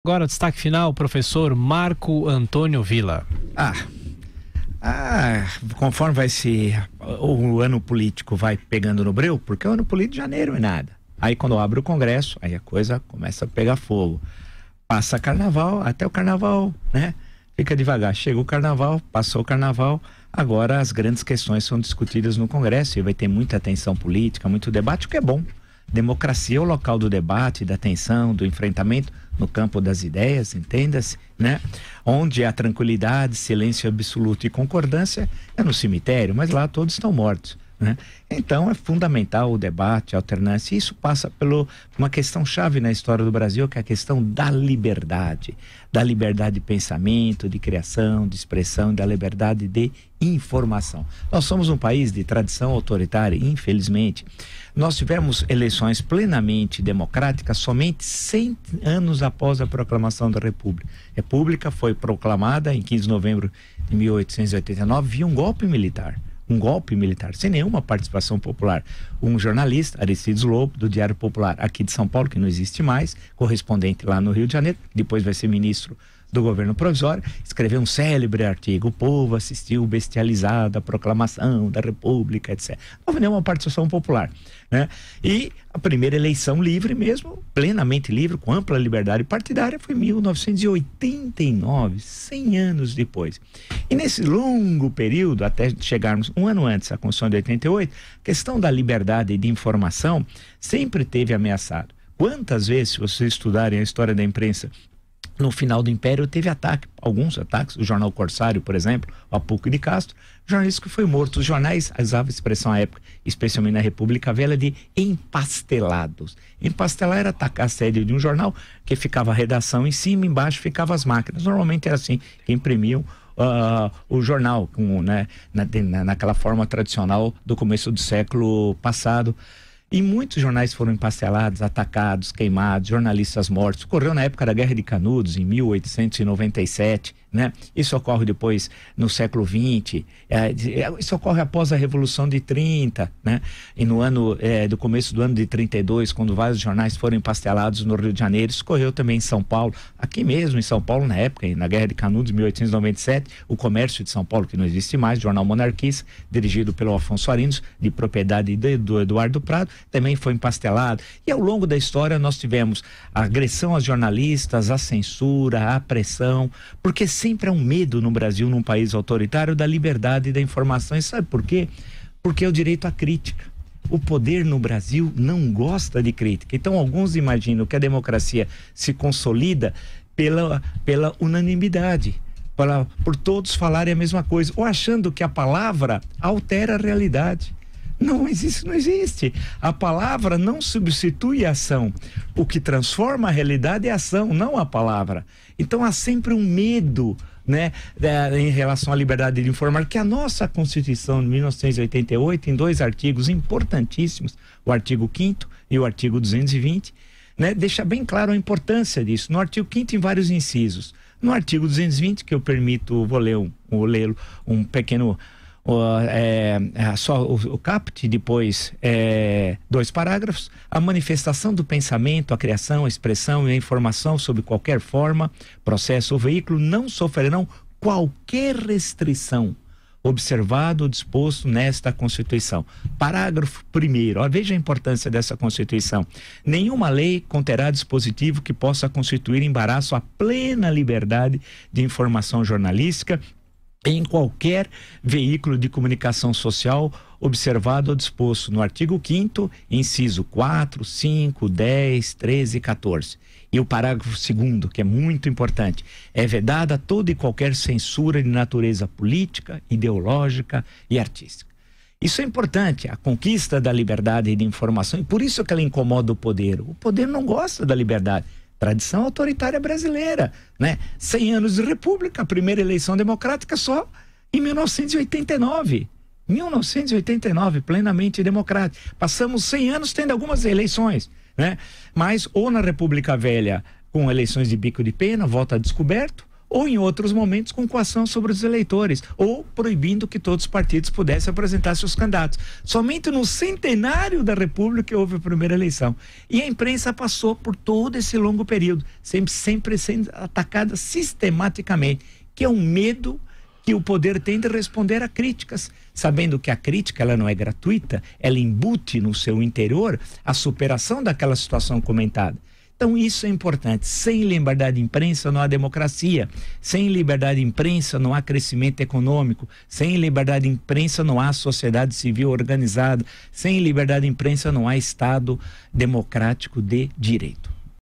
Sem liberdade de imprensa não há democracia
Para Marco Antonio Villa, sem liberdade de imprensa não há democracia.